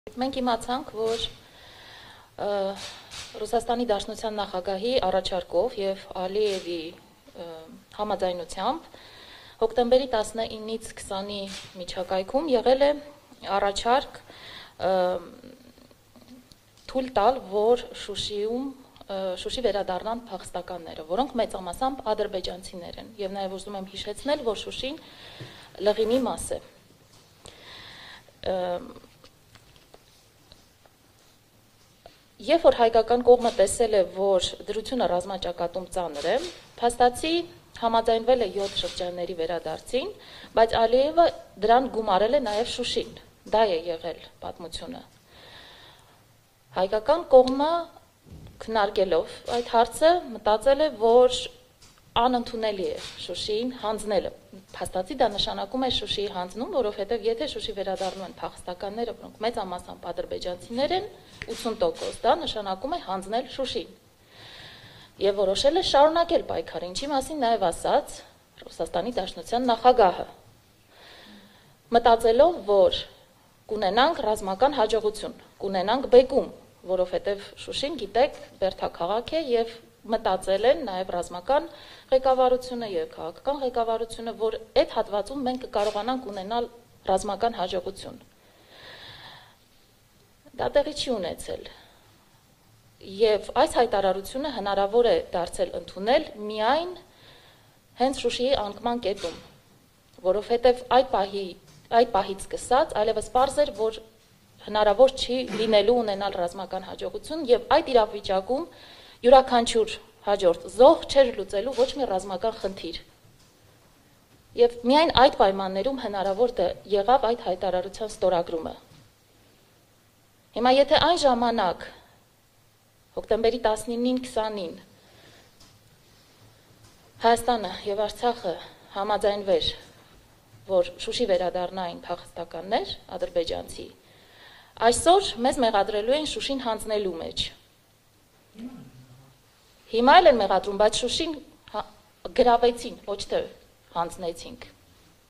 Եթե Շուշի վերադառնային ադրբեջանցի փախստականներ, կնշանակեր հանձնել քաղաքը․ Լենա Նազարյան
Եթե հայկական կողմը համաձայներ, որ Շուշի վերադառնային ադրբեջանցի փախստականներ, կնշանակեր հանձնել քաղաքը․ Շուշին գրավեցին, ոչ թե հանձնեցինք․ Լենա Նազարյան